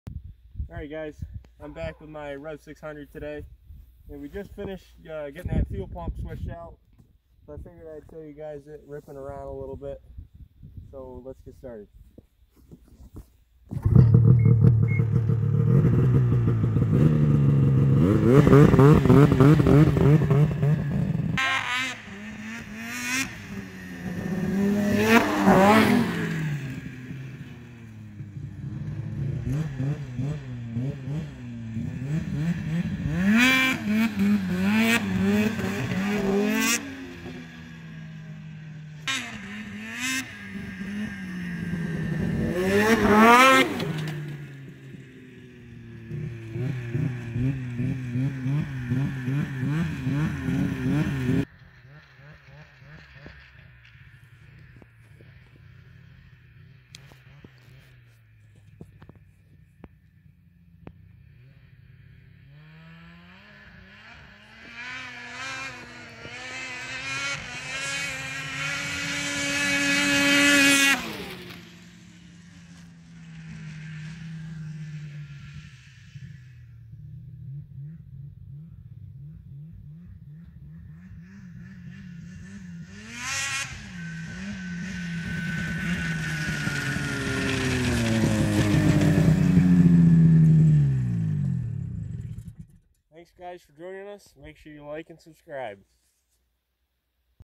03 Rev 600 Ho ride / sound
She has a MBRP can on her. ;) Just replaced fuel pump and got her up and running, and here we are a few short maintenence duties later shes ready to rip! See my channel for tips on rev chassis tips.